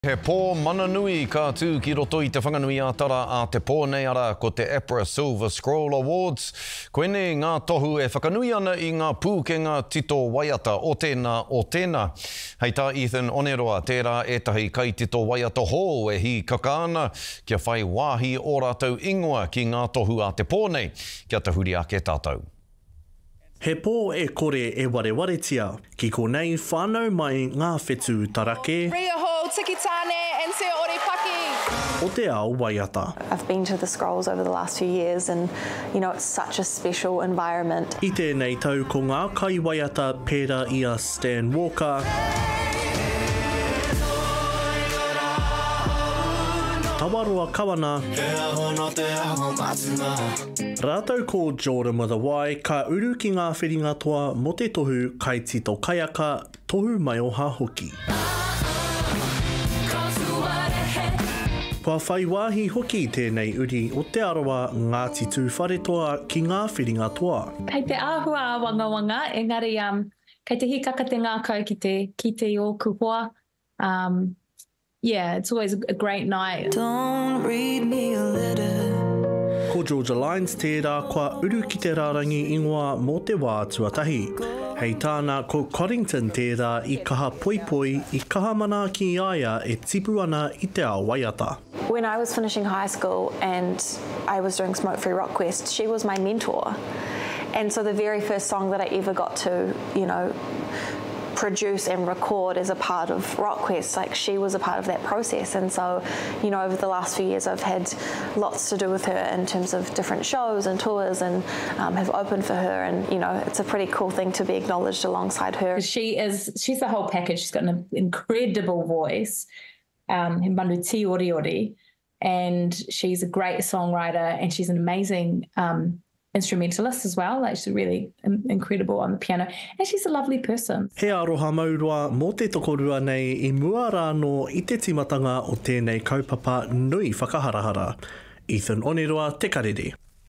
APRA Silver Scroll Awards to take place tonight
TŪREI 08/10/2024

It's one of the biggest nights for revered New Zealand artists that's set to take place tonight in Wellington. The APRA Silver Scroll awards is an opportunity for composers to be recognised for their efforts in song writing. Ethan Oneroa has this story.